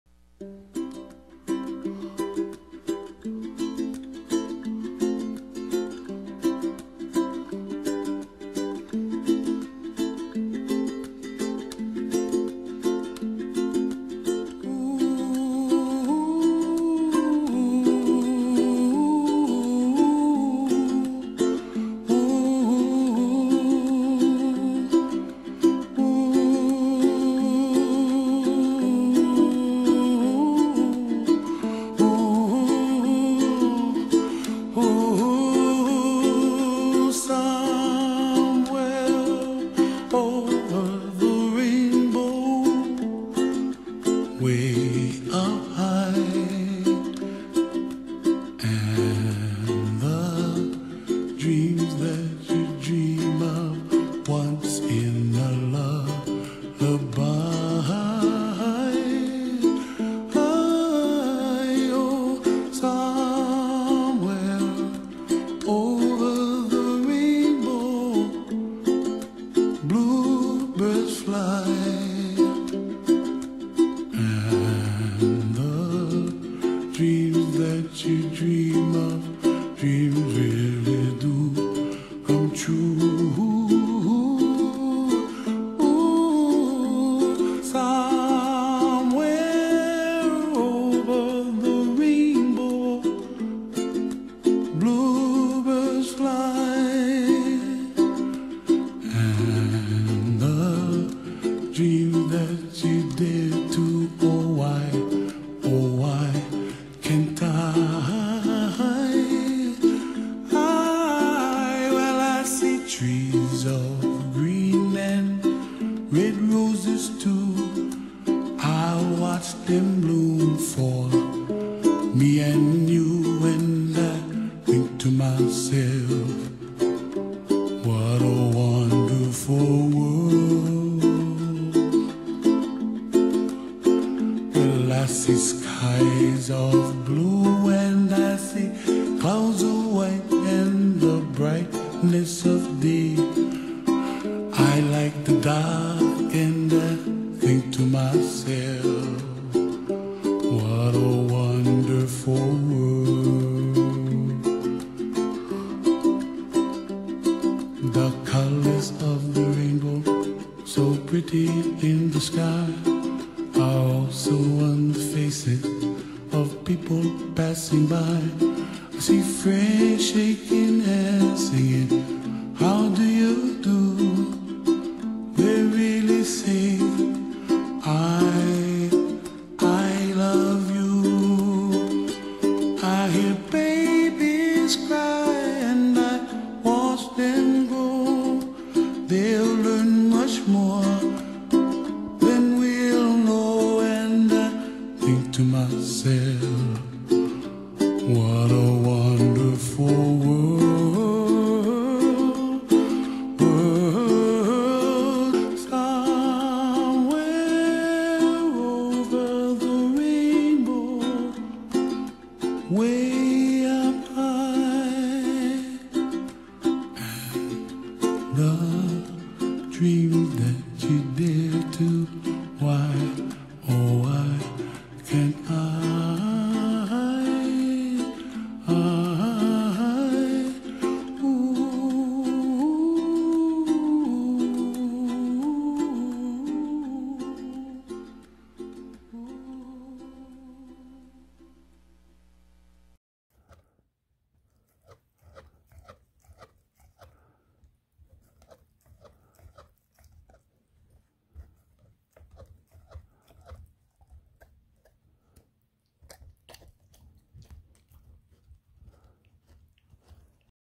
Beefcake Anniversary
Today is a hard day.
My beloved companion Beefcake was laid to rest.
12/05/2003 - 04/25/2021

Licensed to YouTube by Mountain Apple Inc on behalf of Big Boy Record Co.